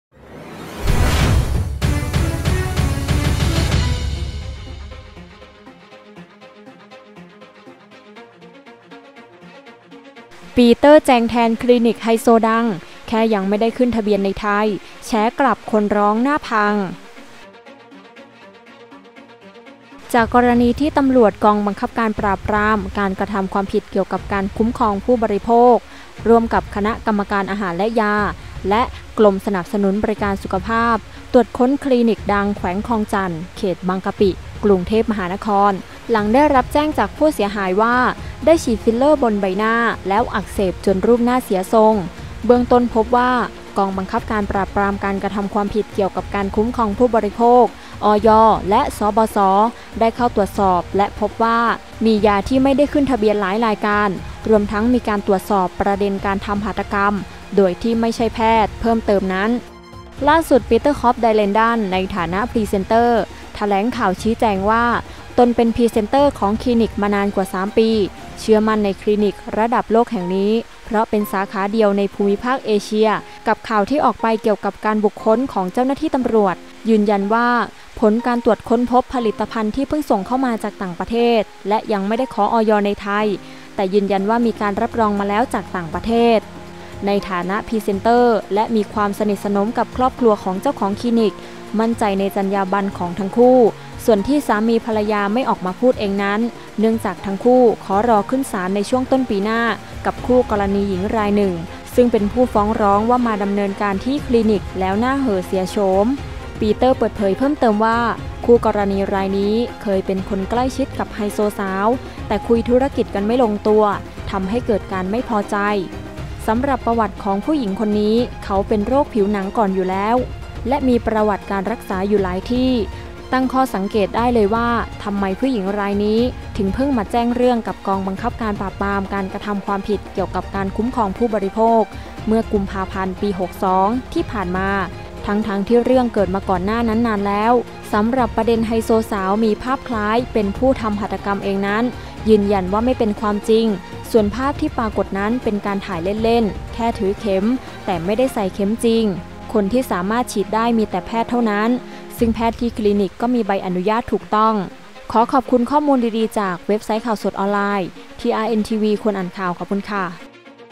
ปีเตอร์ แจงแทน คลินิกไฮโซดัง แค่ยังไม่ขึ้นทะเบียนในไทย แฉกลับคนร้องหน้าพัง!
ล่าสุด ปีเตอร์ คอร์ป ไดเรนดัล ในฐานะพรีเซ็นเตอร์ แถลงข่าวชี้แจงว่า ตนเป็นพรีเซ็นเตอร์ของคลินิกมานานกว่า 3 ปี เชื่อมั่นในคลินิกระดับโลกแห่งนี้ เพราะเป็นสาขาเดียวในภูมิภาคเอเชีย กับข่าวที่ออกไปเกี่ยวกับการบุกค้นของเจ้าหน้าที่ตำรวจ ยืนยันว่า ผลการตรวจค้นพบผลิตภัณฑ์ใหม่ที่เพิ่งส่งเข้ามาจากต่างประเทศ และ ยังไม่ได้ขอ อย.ในไทย แต่ยืนยันว่ามีการรับรองมาแล้วจากต่างประเทศ

รับชม ข่าวสารใหม่ๆ ที่ช่อง คนอ่านข่าว - TRNTV